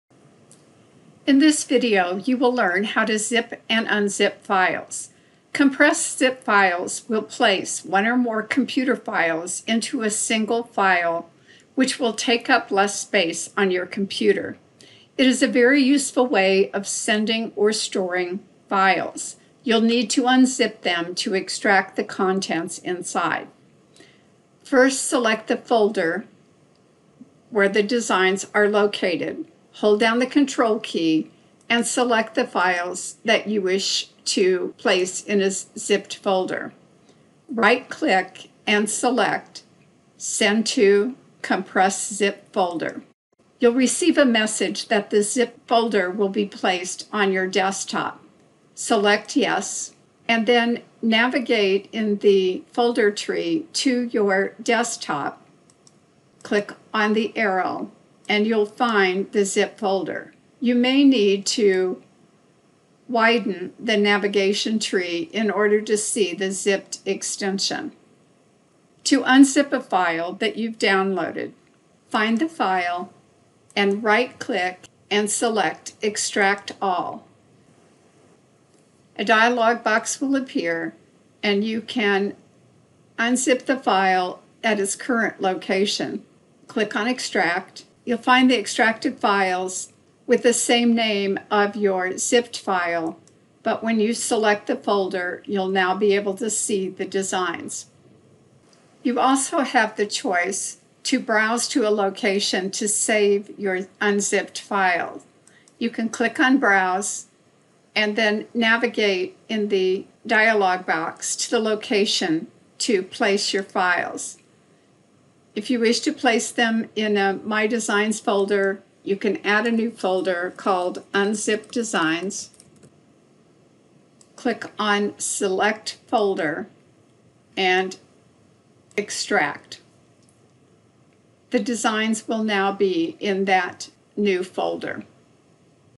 2 09 Zipping and Unzipping Designs
Learn how to zip and unzip groups of designs in Embroidery Library in BERNINA Embroidery Software 9.